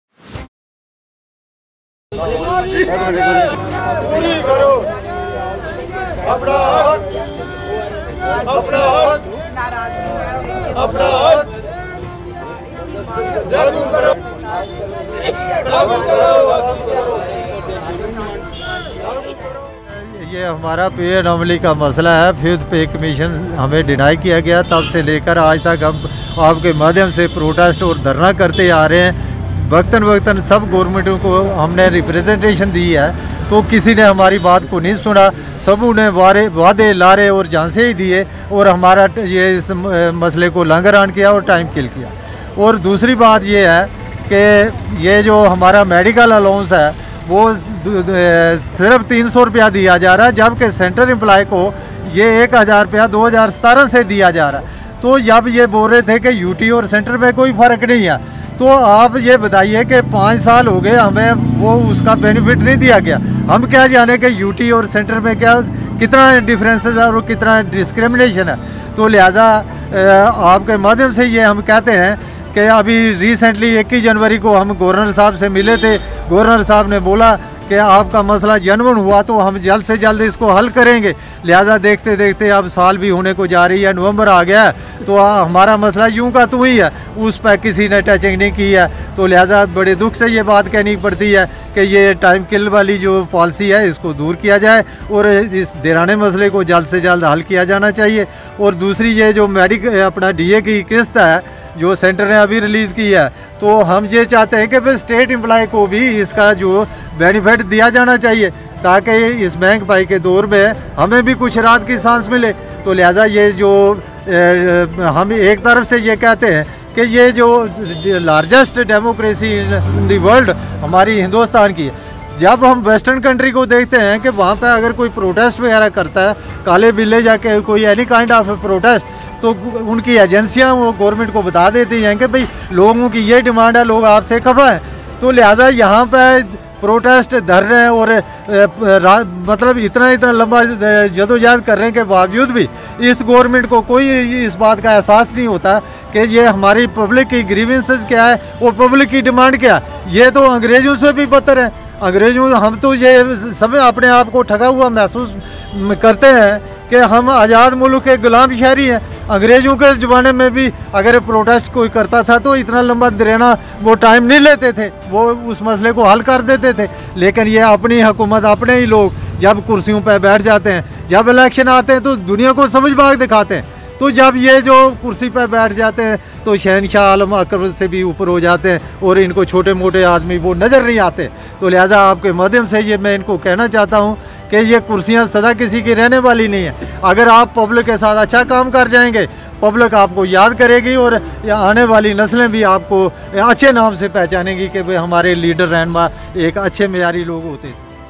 All J&K retired police NGO & ORS Forum held Protest in jammu
सेवानिवृत्त पुलिस कर्मियों ने अपनी मांगों को लेकर जम्मू में किया प्रदर्शन, जम्मू कश्मीर प्रशासन पर लगाया उनके मुद्दों को नजरंदाज करने का आरोप